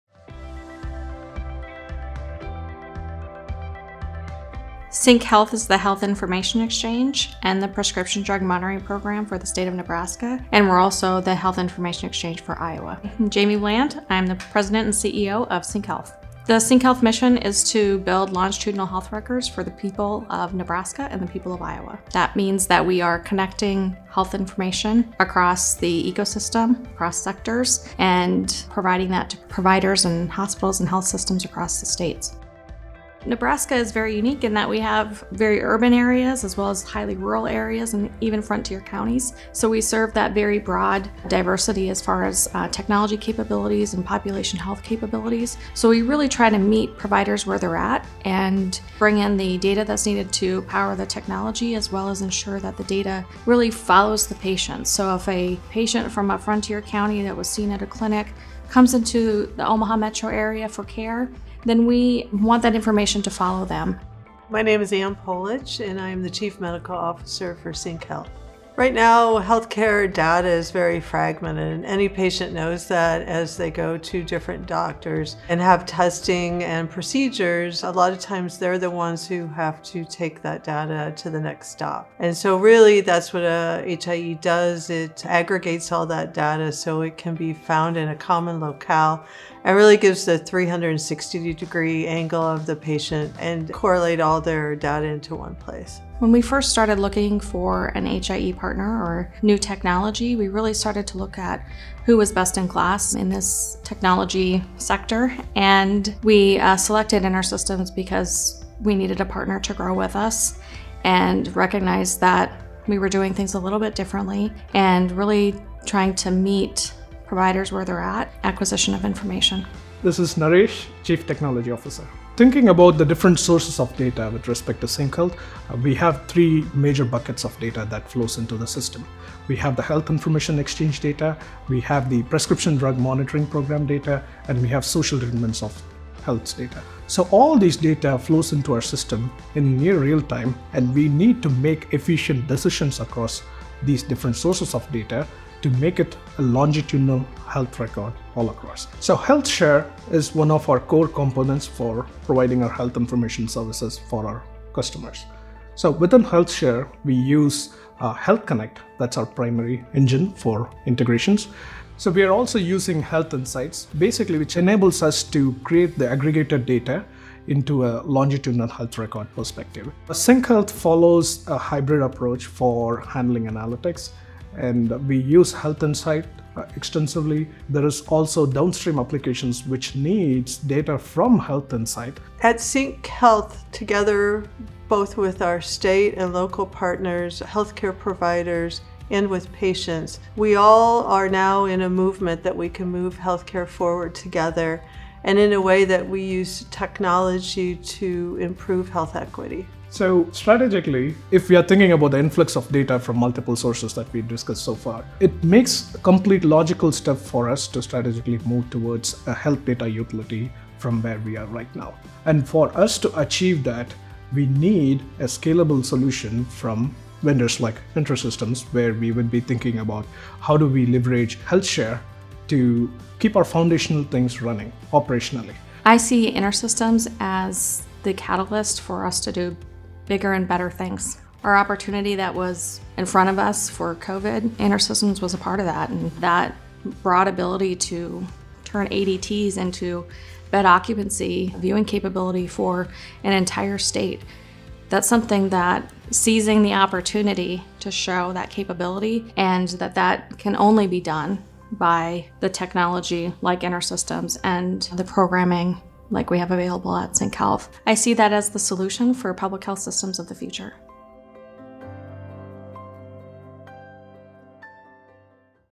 Creating Multi-Source, Longitudinal Health Records for Analytics and Better Decision Making
As a major component in the public health system for Nebraska and Iowa, CyncHealth leverages InterSystems HealthShare Health Connect and Health Insight to build longitudinal health records for diverse populations. Learn why CyncHealth considers InterSystems as “the catalyst to do bigger and better things.”